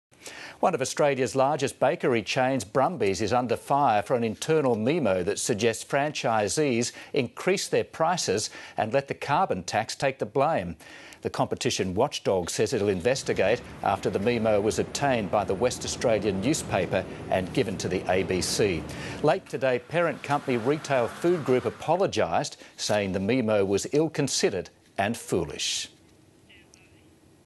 Brumby's criticised for carbon tax memo
Bakery chain Brumby's is under fire for a leaked internal memo that suggests franchisees increase their prices and 'let the carbon tax take the blame'.